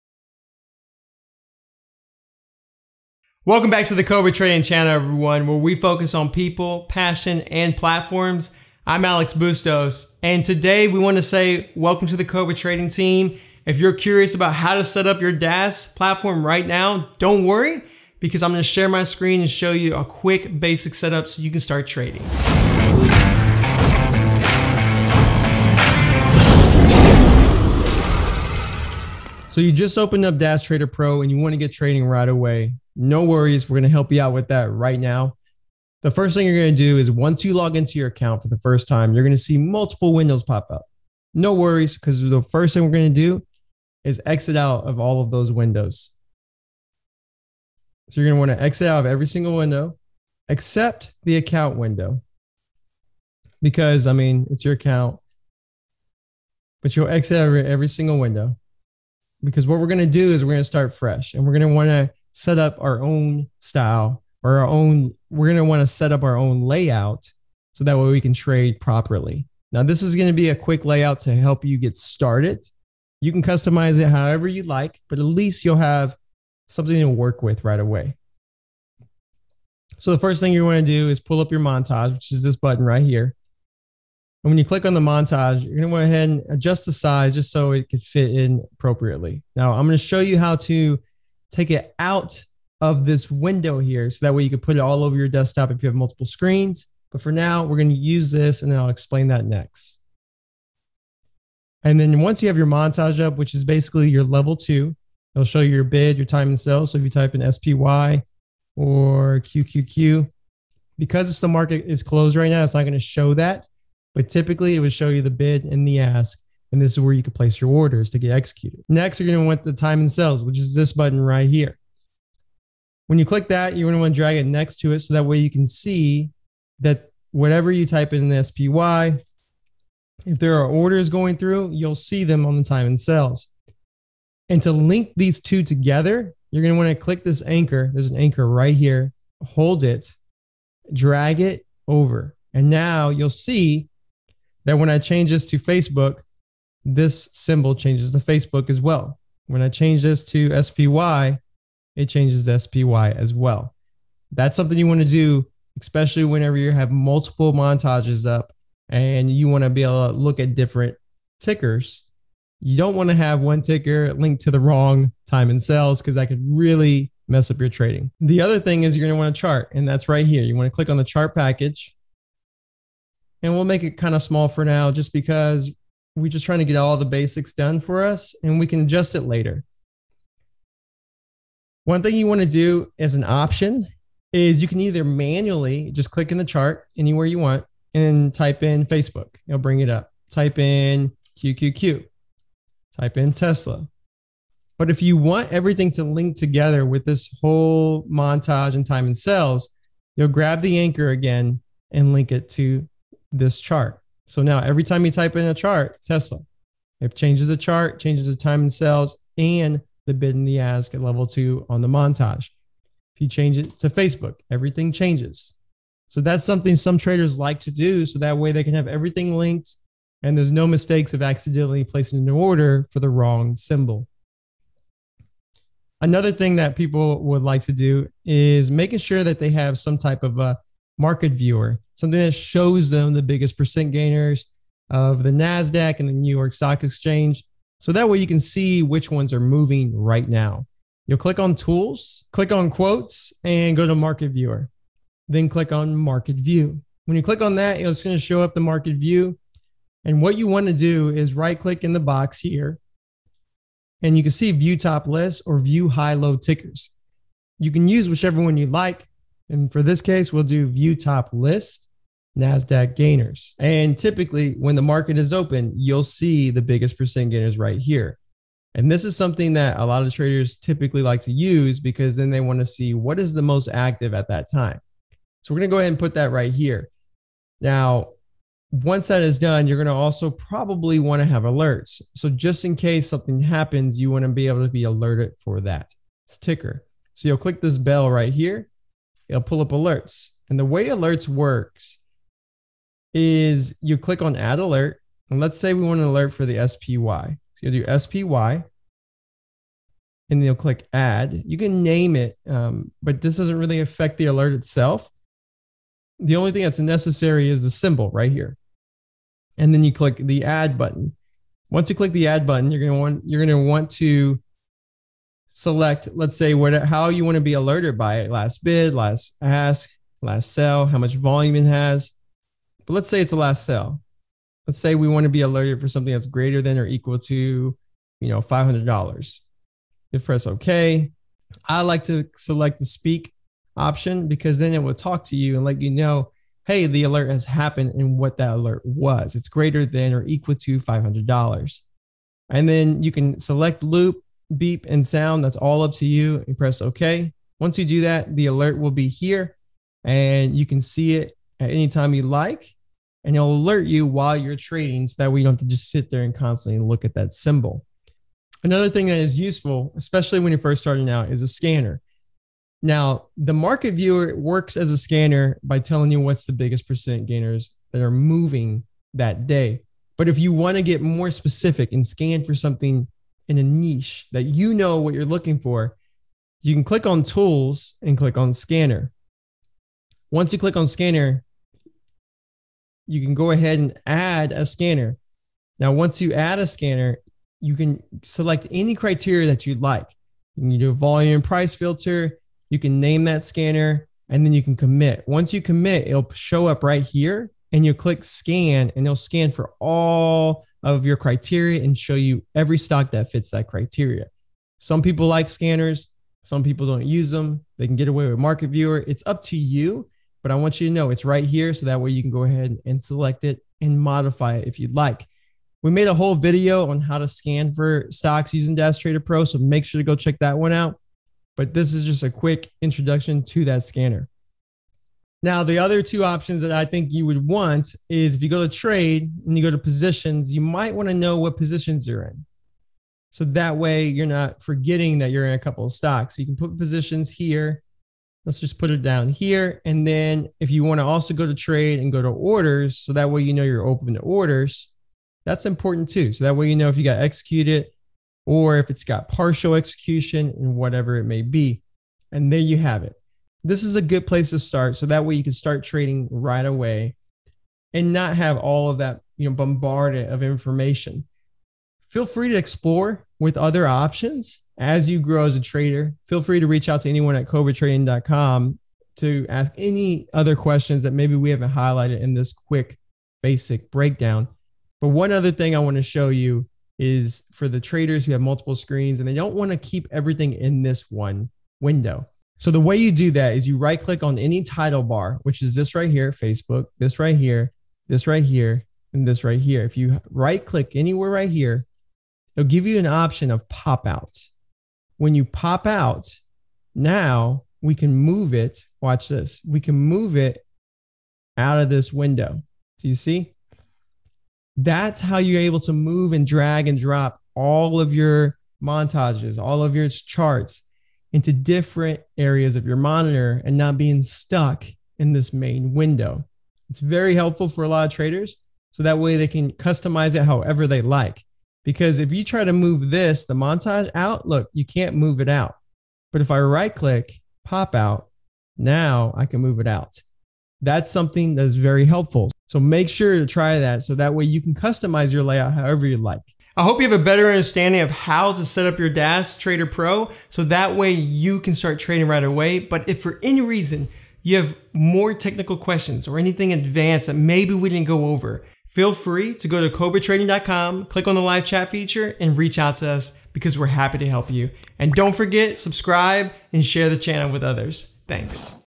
How To Setup Your Das Trader Pro Trading Software
Today Alex will show you how to set up your Das Trader Pro Software so you can be on your way to trading the markets.